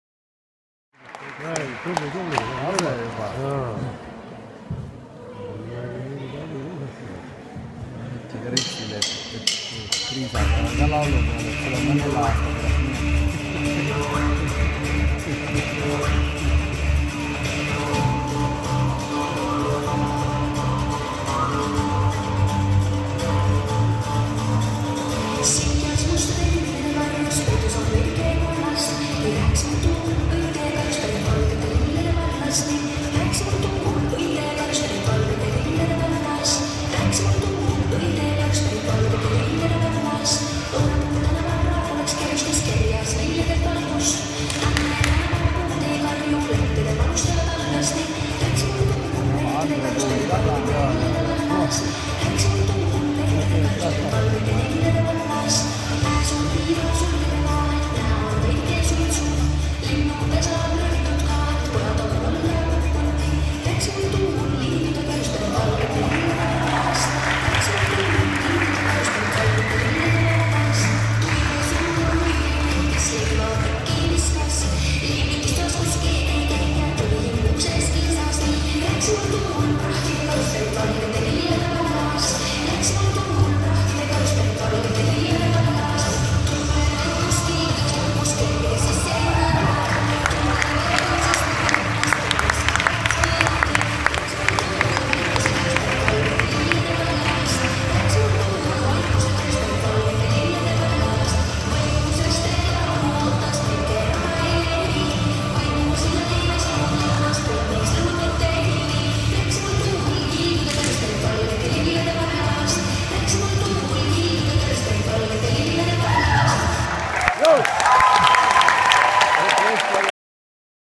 Parksepa KK 11A Klass - Lillekülvajad 2015
Teist aastat järjest pani meie klass tantsufestivali kinni, Lillekülvajad!